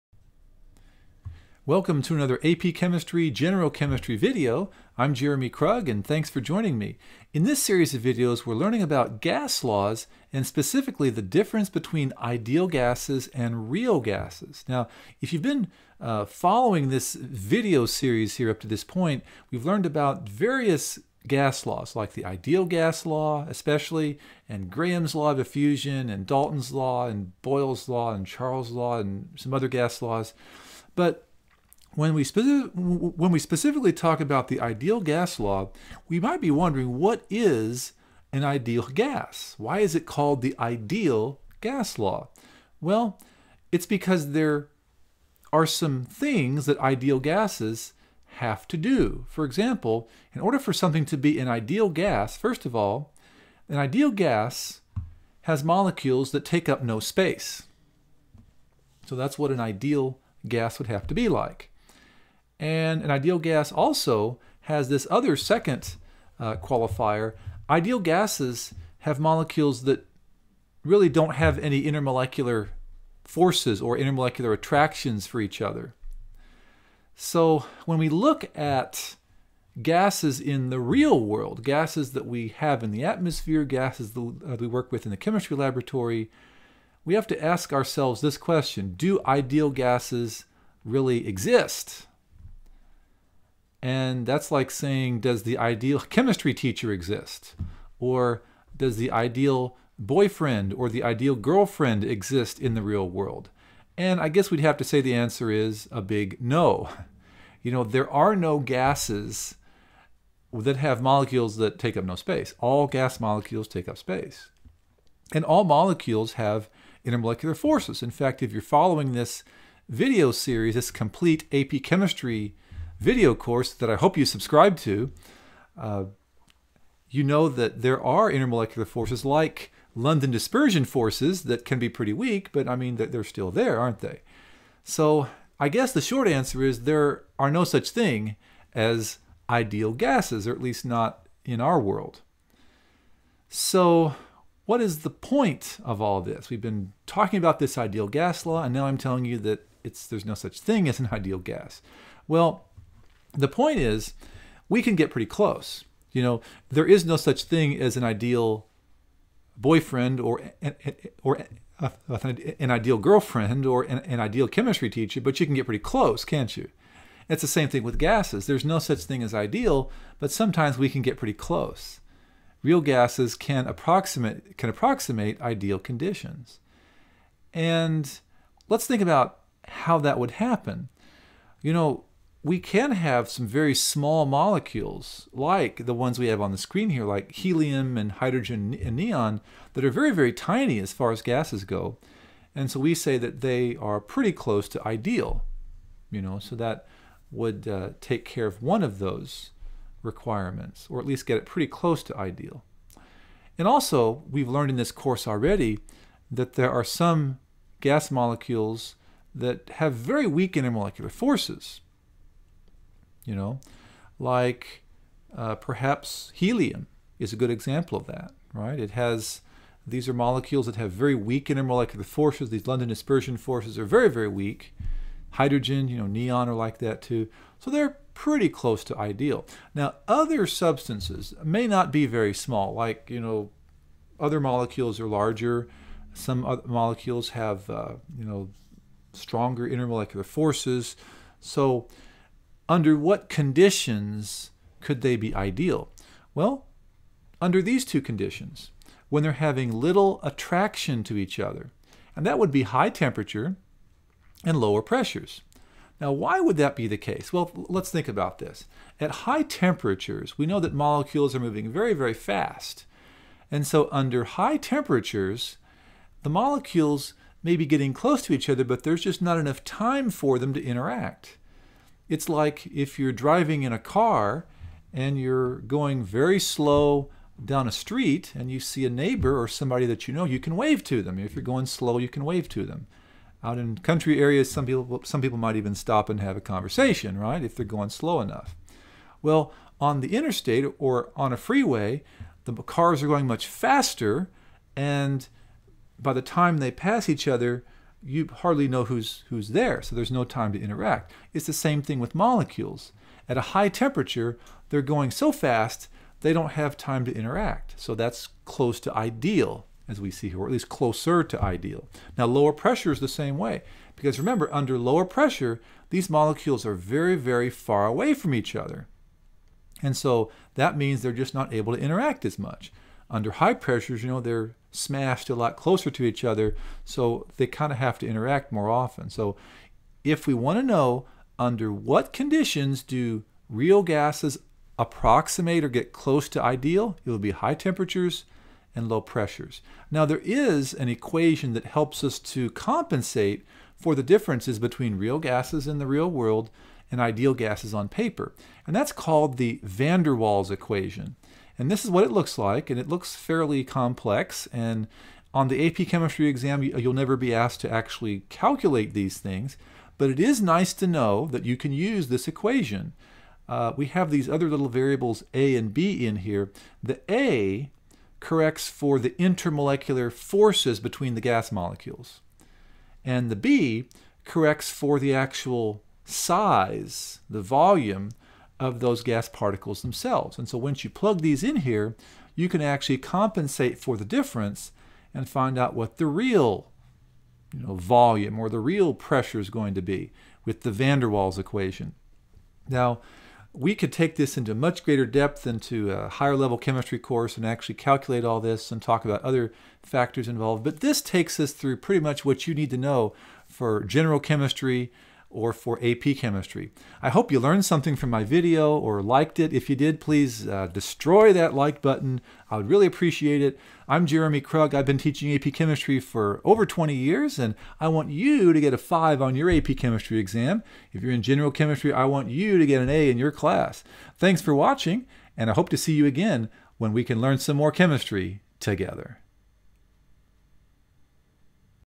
Real vs Ideal Gases & the van der Waals Equation - AP Chemistry Complete Course - Lesson 24.4
In this video, Mr. Krug describes the differences between real and ideal gases.  He discusses the conditions under which a real gas can approximate ideal conditions and introduces the van der Waals equation for real gases.